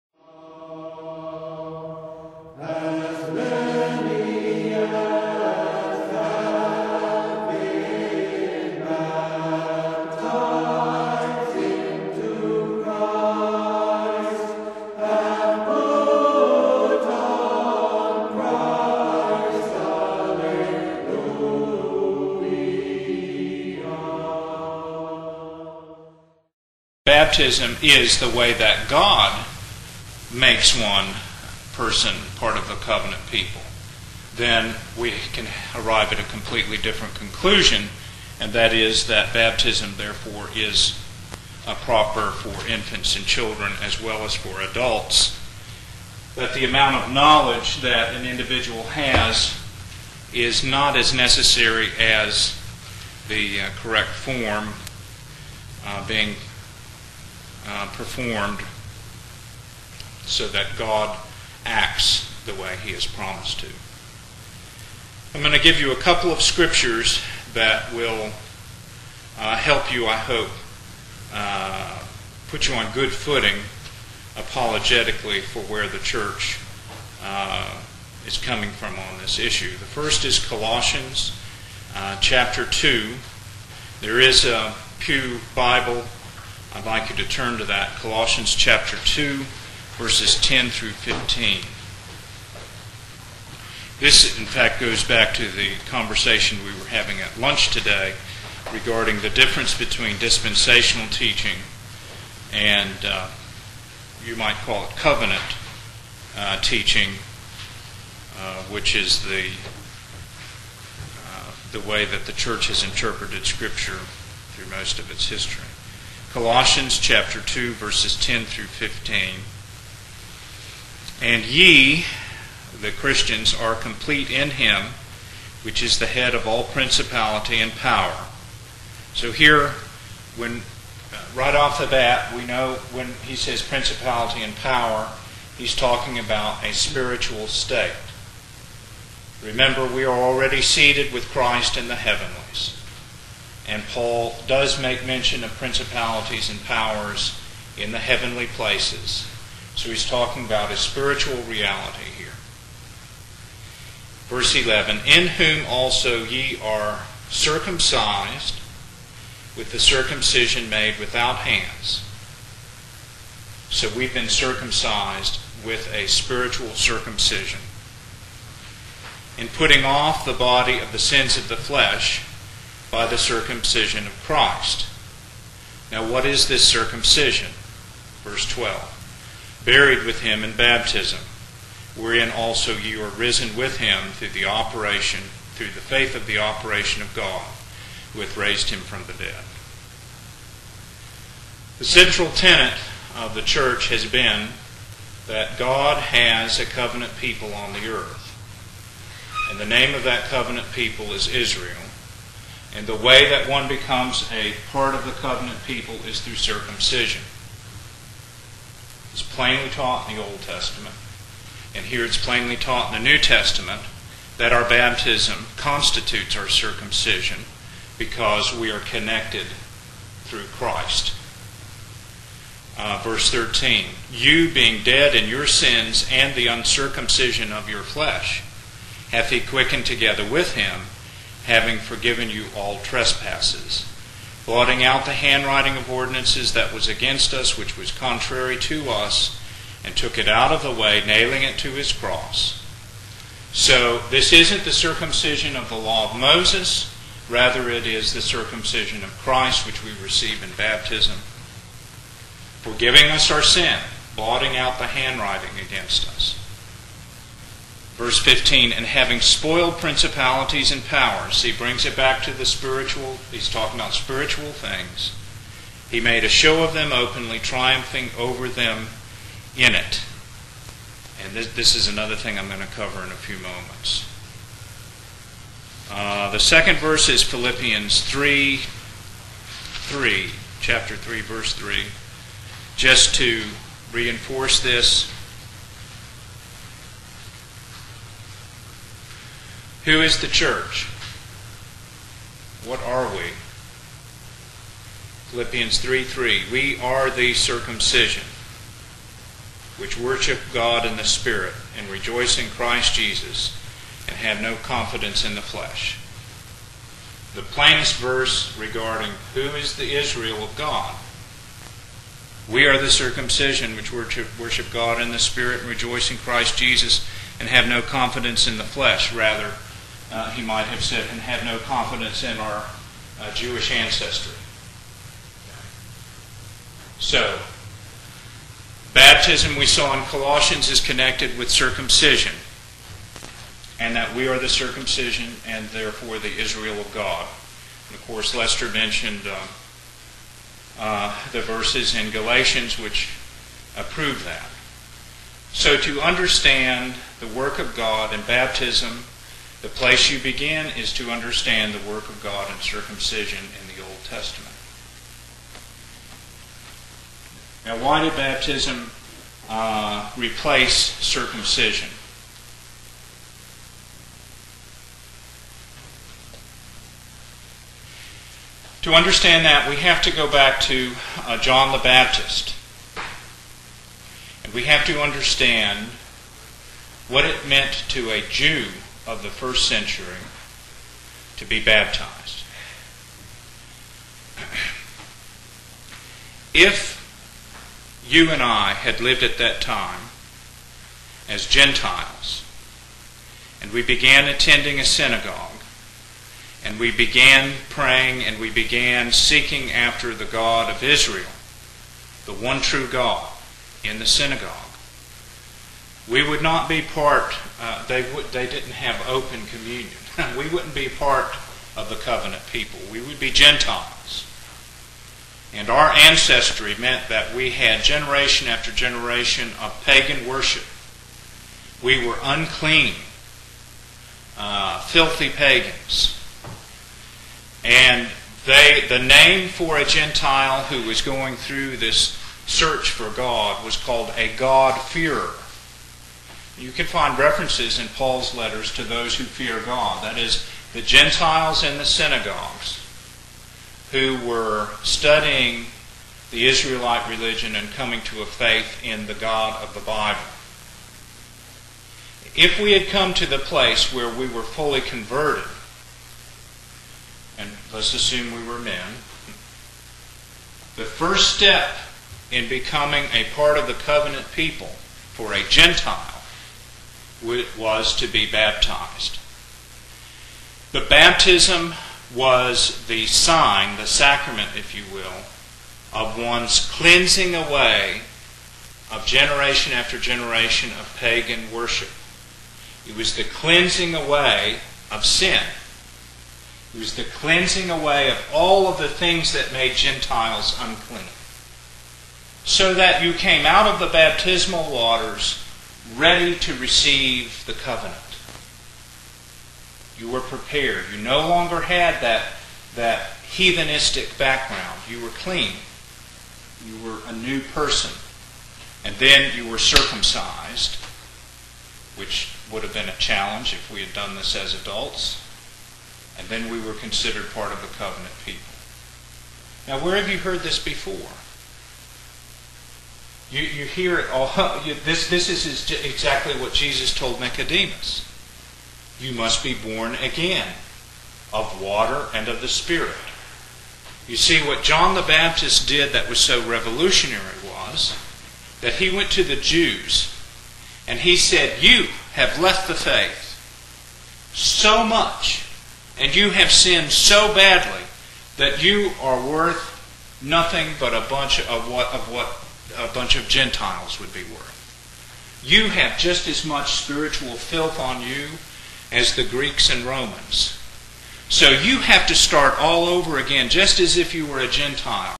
Baptism Lecture Part A1
This is the first part (Part A1) of a lecture on the Sacrament of Holy Baptism given by Archbishop Scott McLaughlin.  Archbishop McLaughlin is the Presiding Bishop of the Orthodox Anglican CHurch, Metropolitan of the Orthodox Anglican Communion, and Chancellor of St Andrew's Theological College and Seminary.